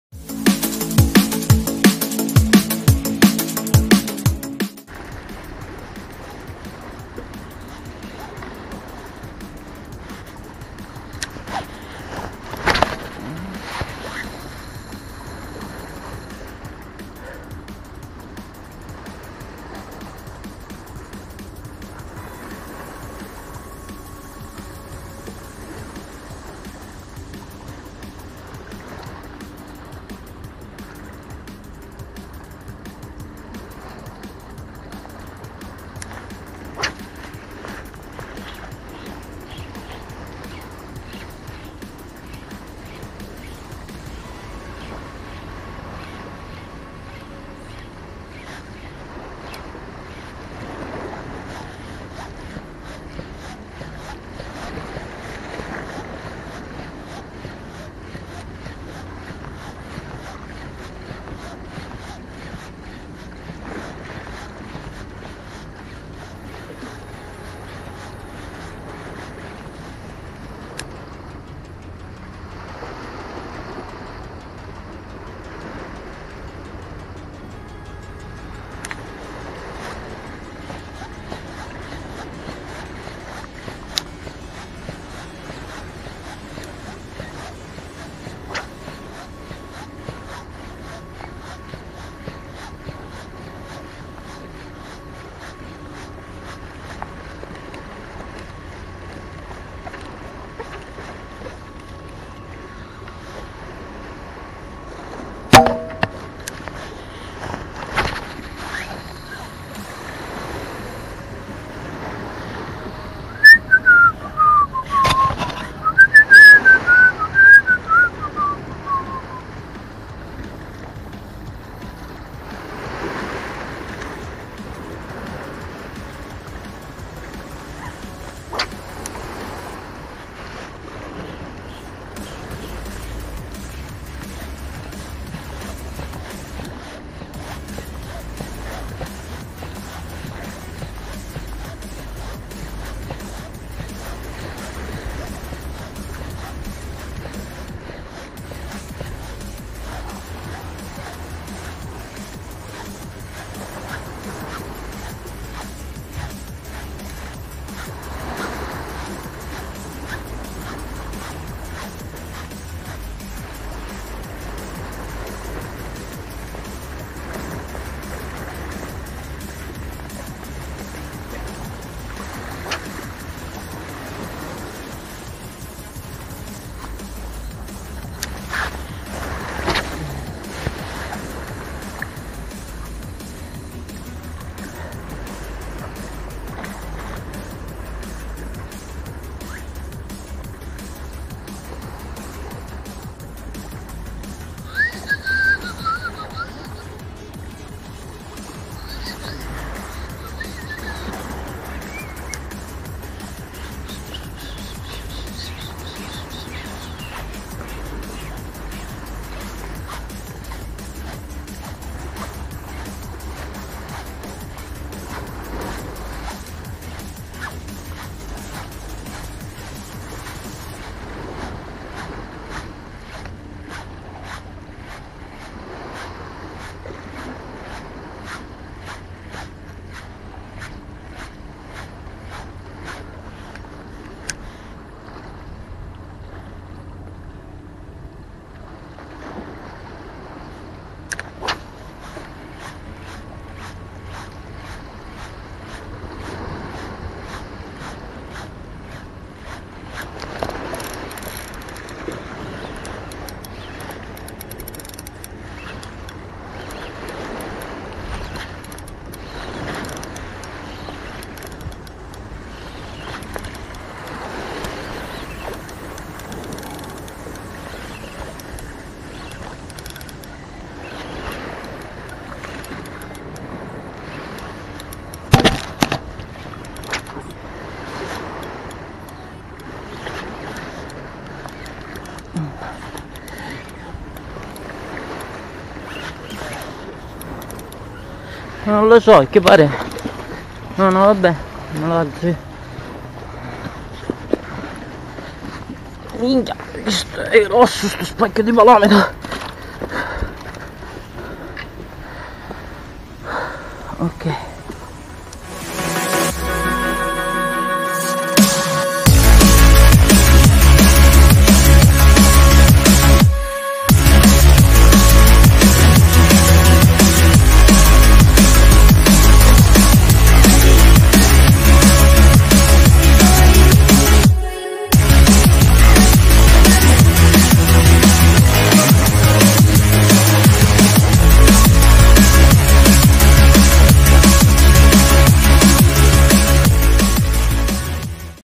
Poker di palamite
Bellissima mattinata molto divertente, svolta con il rilascio di un barra e una ricciola, ma conclusa con 4 bellissime palamite. Zenaqsoniomuthos100m#shimanostella swb14000